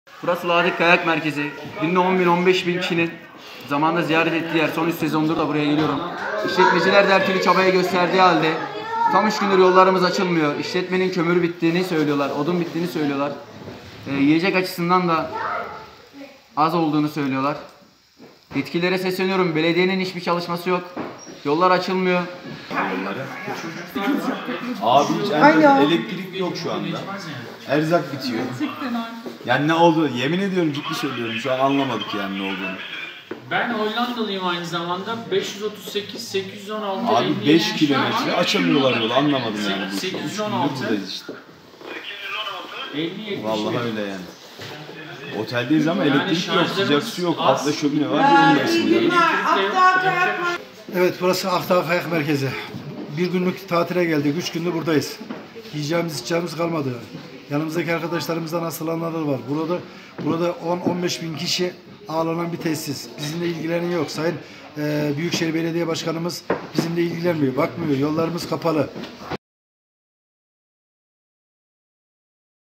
Vatandaşlar 3 gündür otelde mahsur kaldılar! Elektrik, yiyecek, yakacak yok!
Samsun'un kış turizm merkezi olan Ladik Akdağ'da tatilciler adeta perişan oldu. 1 günlük tatil yapmaya gelen vatandaşlar 3 gündür otelde mahsur kalırken elektrik, yiyecek ve yakacak konusunda sıkıntılar yaşanıyor.